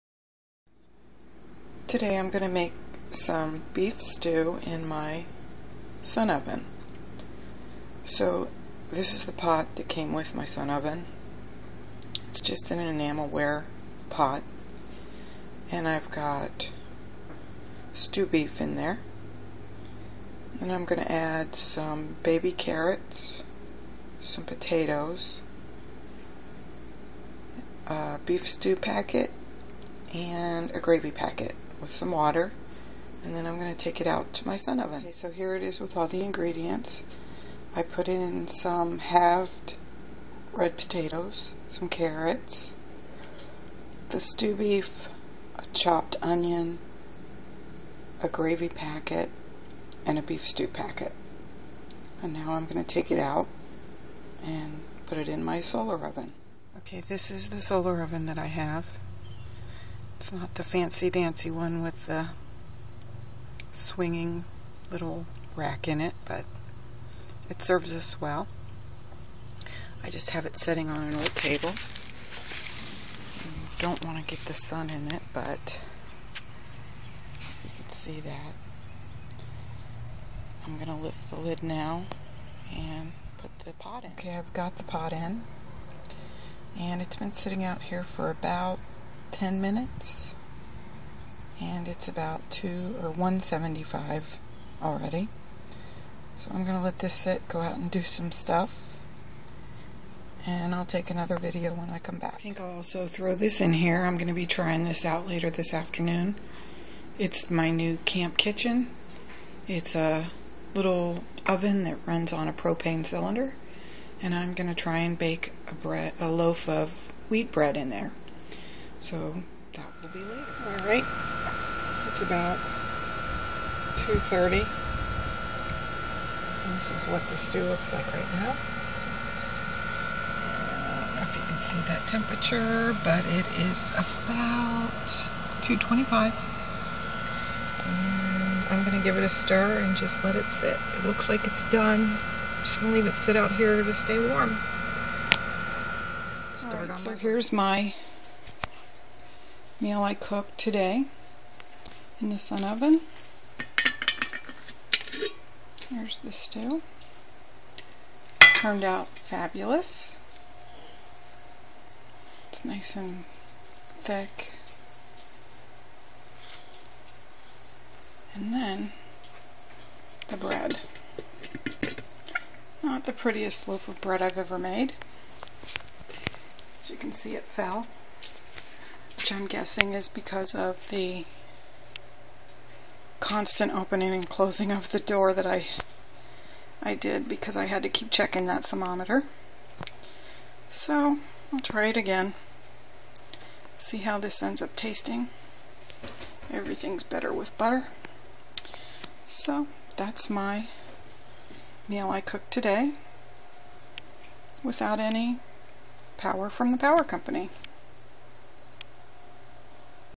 SOS Solar Oven Dinner
This is the beef stew that I cooked in my solar oven. I put it in around 9:30am, left to go to the movies and checked it around 2:30 and it was done.